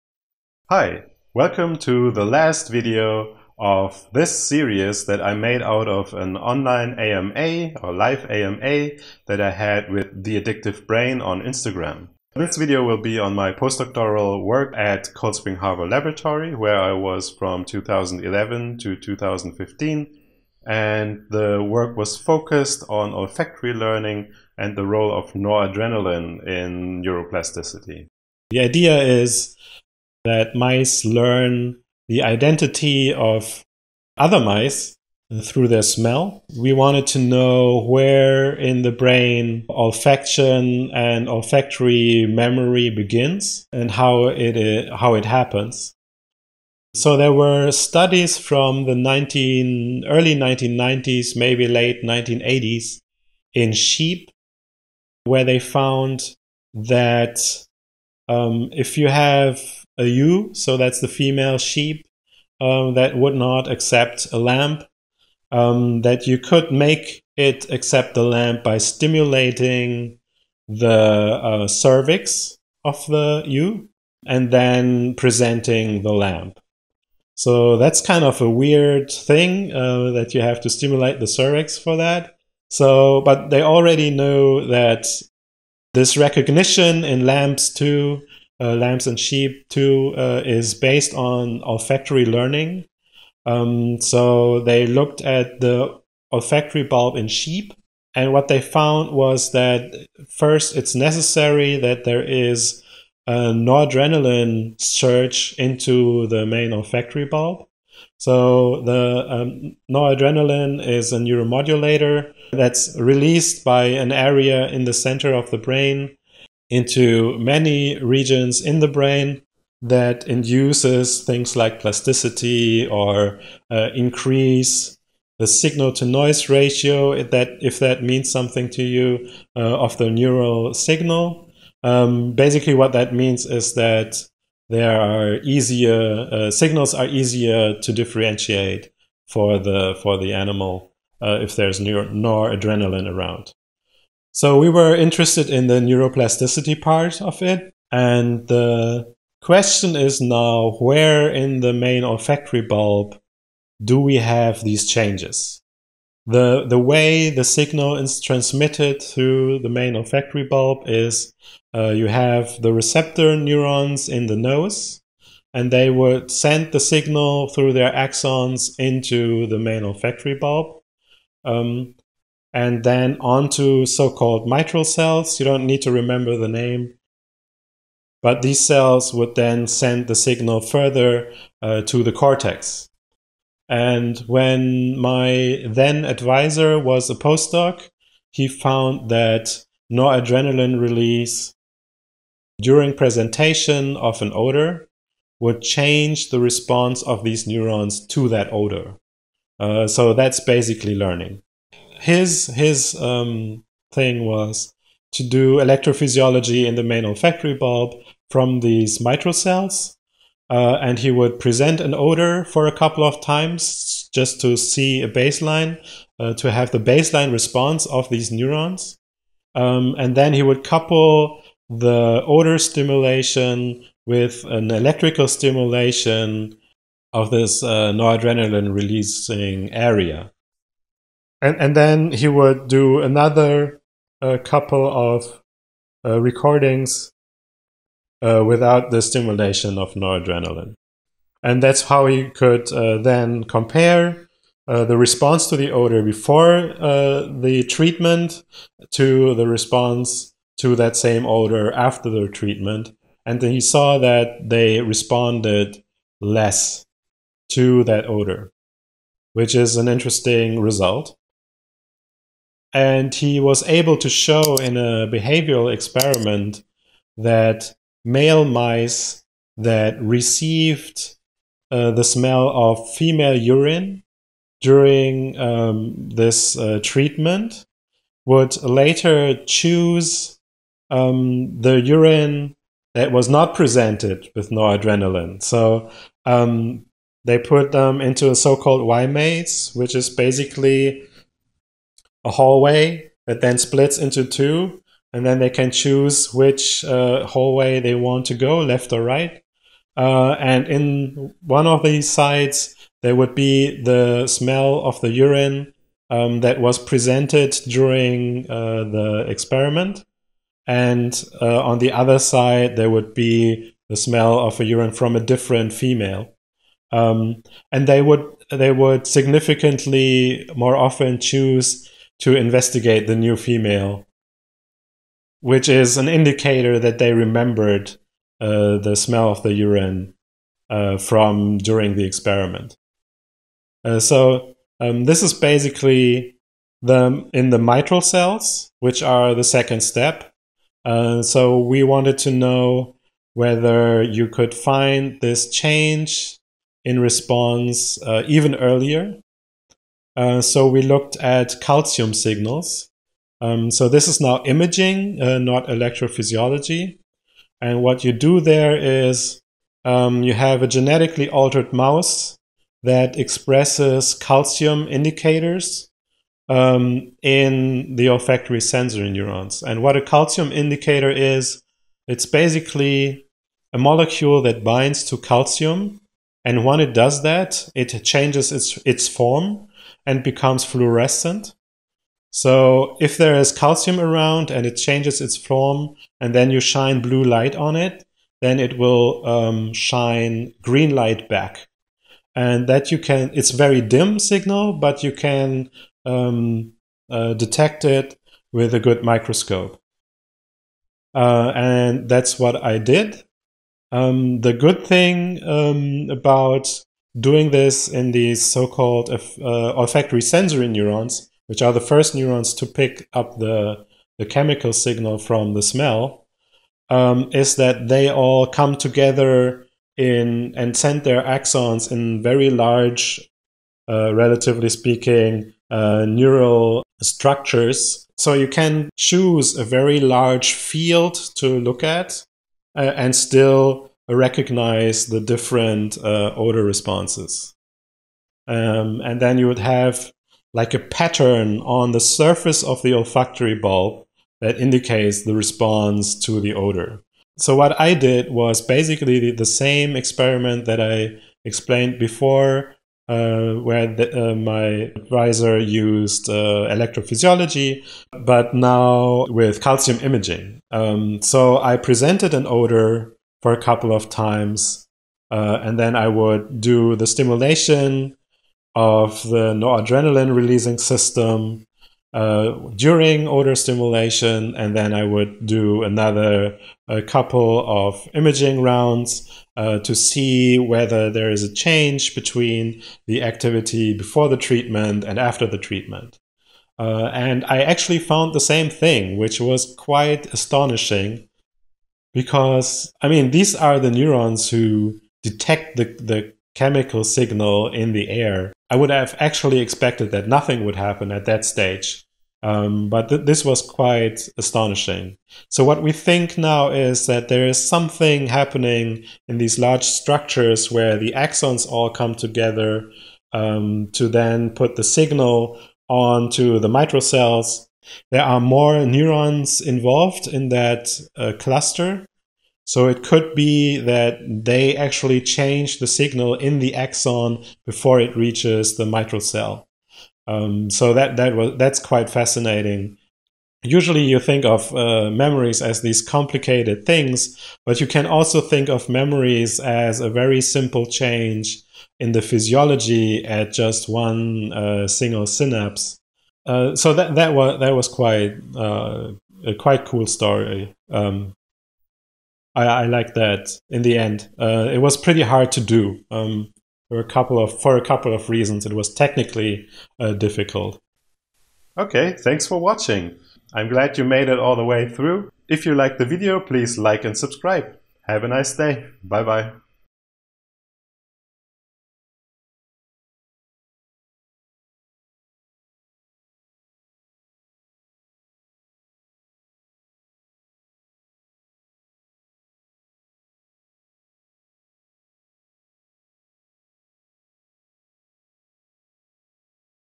Postdoc study: mice learning smells - AMA part 4
As a postdoc I studied how mice learn smells. We knew that in order for mice to recognize each other, they needed to learn each other's smells. Already in the earliest structure in the olfaction processing pathway, the main olfactory bulb, physiological changes were known to be involved in this learning process. I used calcium imaging to see where in the main olfactory bulb, changes would occur. I found that already the signal entering the bulb was changed!